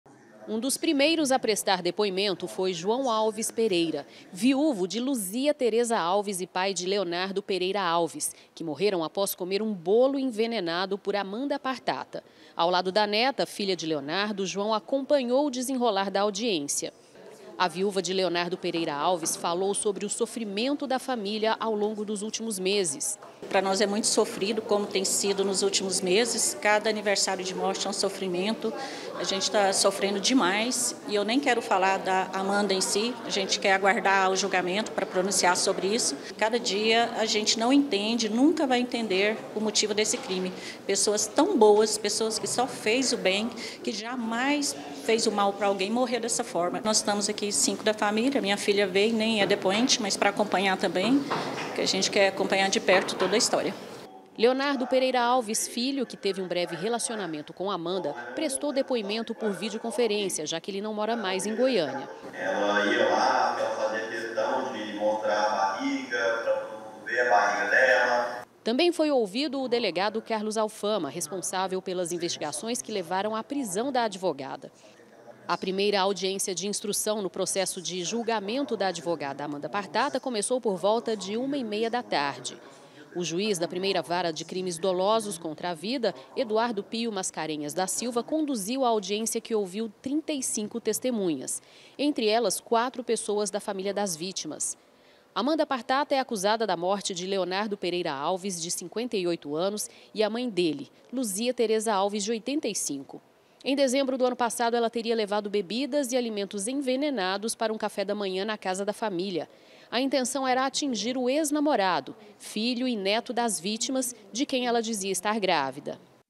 BOLO ENVENENADO: PRIMEIRA AUDIÊNCIA ACONTECE NA JUSTIÇA
ACONTECEU HOJE PRIMEIRA AUDIÊNCIA NA JUSTIÇA DO CASO  AMANDA PARTATA. ELA É ACUSADA DE MATAR O EX-SOGRO E A MÃE DELE,  ENVENENADOS COM UM BOLO DE POTE. A MÔNICA NOVAES ACOMPANHOU E TRAZ AS INFORMAÇÕES PRA GENTE.